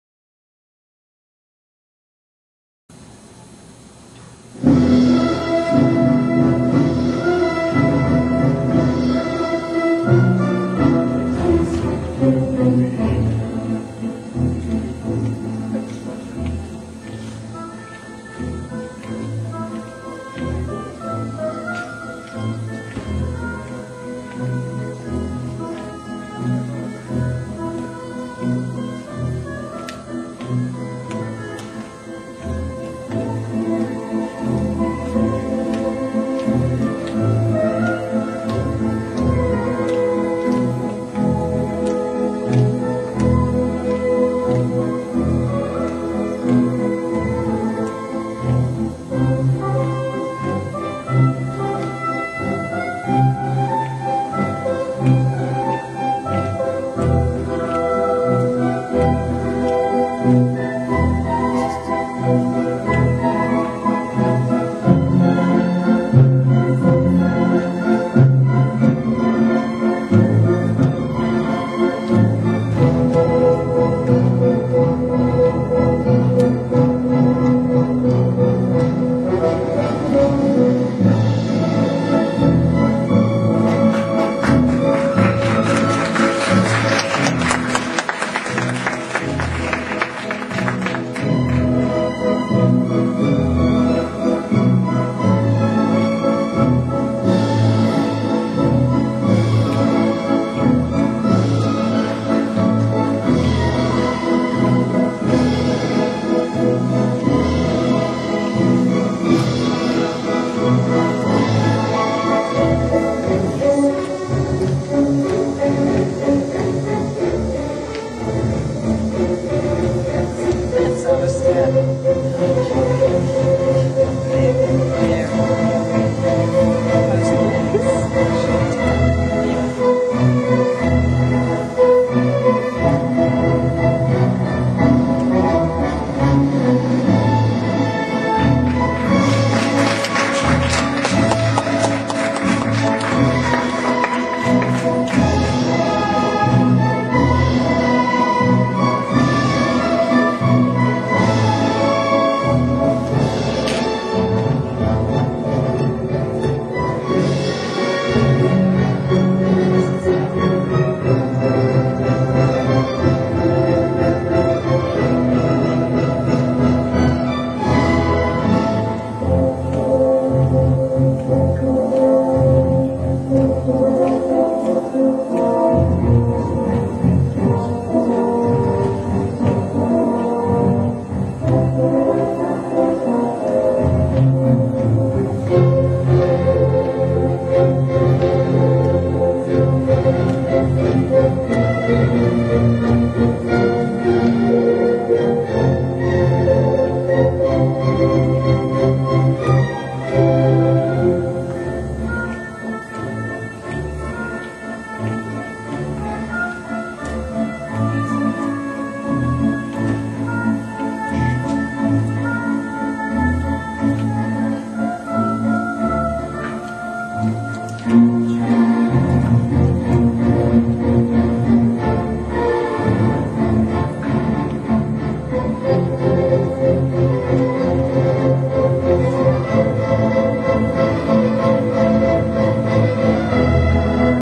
Diplomats perform the Polonaise
2008 Diplomats perform at the farewell dinner located at Wake Forest